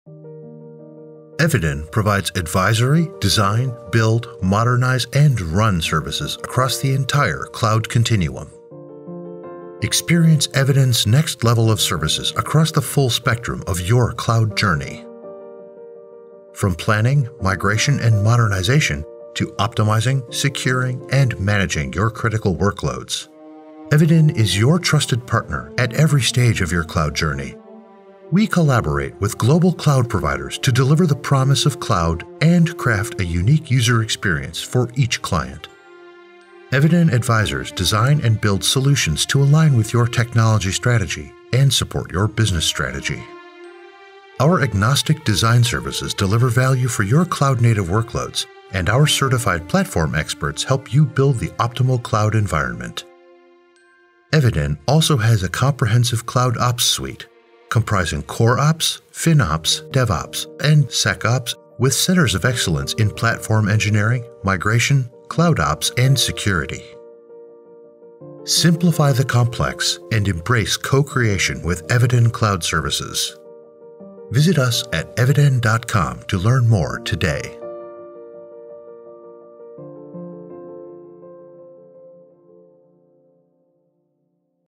The Eviden Cloud Continuum
Eviden provides Advisory, Design, Build, Modernize and Run services across the entire Cloud Continuum - from planning, migrating and modernizing, to optimizing, securing and managing your critical workloads.